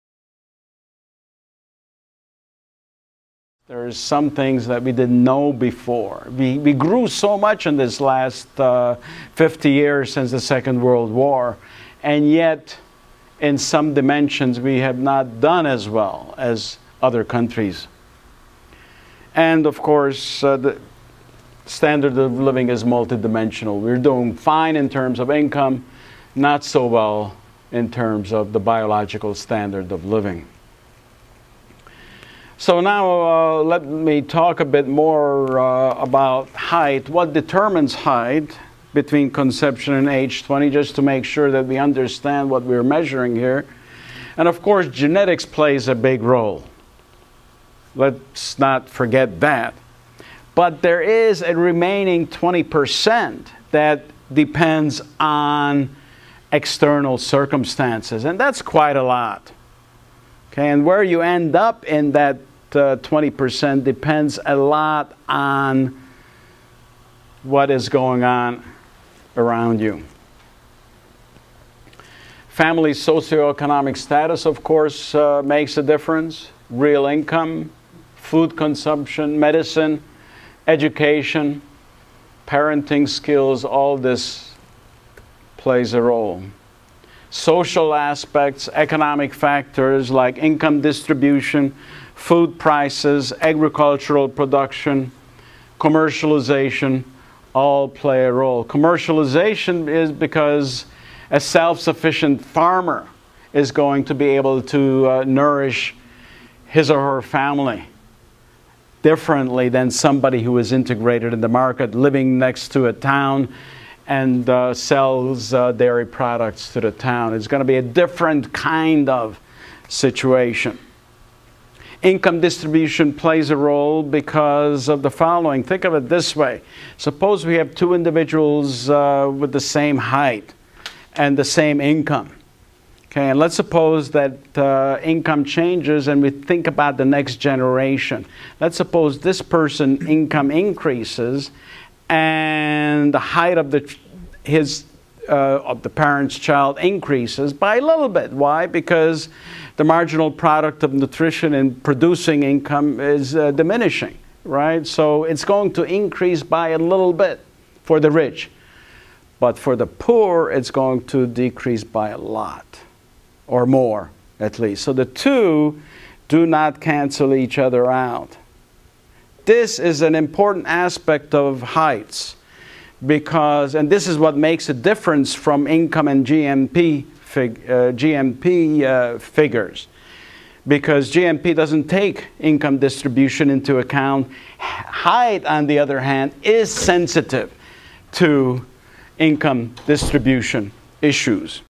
"Economics and Human Biology: Why Are Americans Shorter and Fatter?"
Dr. John Komlos of the University of Munich gave the Allen Starling Johnson, Jr. Distinguished lecture on October 28. He discussed how height can reflect society's well-being.